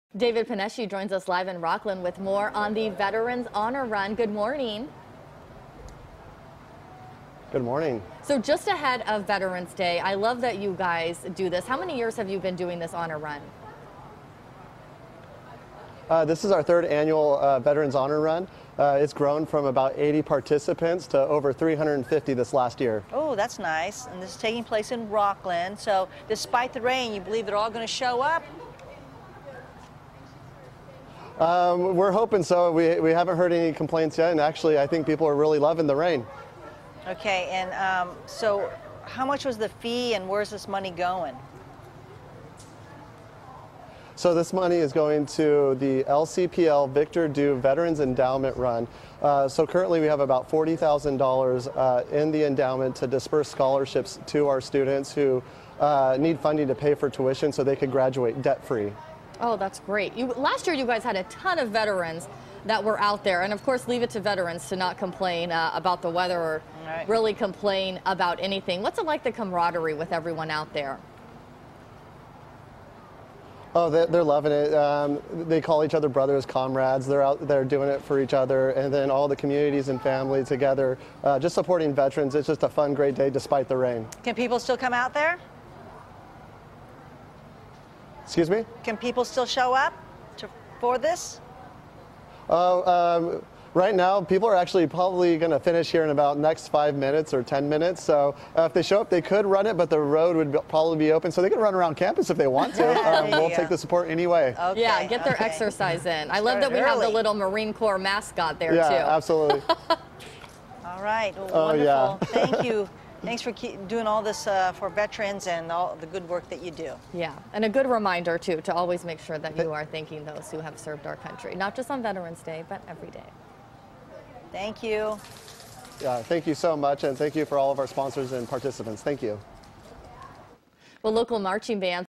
Veterans Honor Run
We’re learning more about the 3rd annual Veterans Honor Run with through participation and sponsorships will allow Jessup to alleviate the cost of education for our veteran and Gold Star Family students.